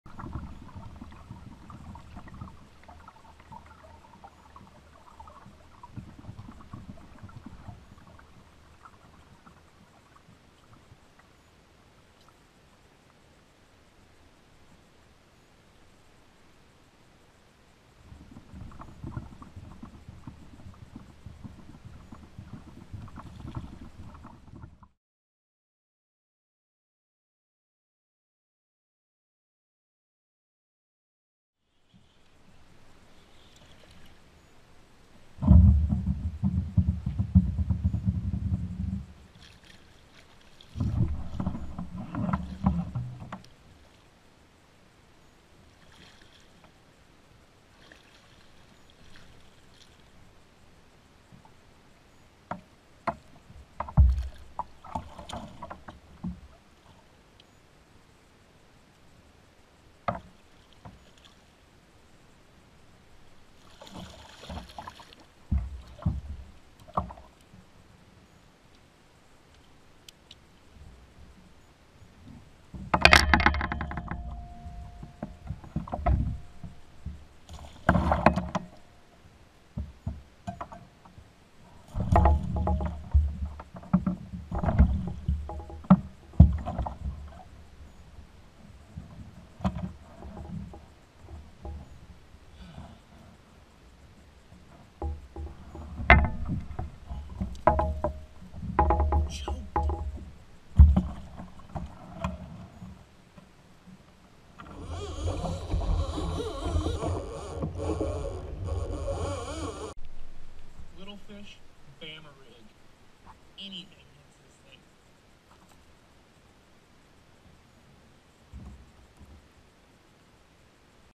Kayak Fishing : Striper on the Pro-Cure covered Big Hammer A-Rig
Kayak Fishing with Big Hammers Swimbaits put on a Alabama rig and then Pro-Cure Sauce rubbed in for scent and the fish came. The kayak offers an excellent platform for trolling for the Striped bass and there is no better kayak then the Hobie.

Check out CCKF.NET for more action.
Check out pro-cure.com for some scent and Swimbait.com to order some Big Hammers.

CCKF.NET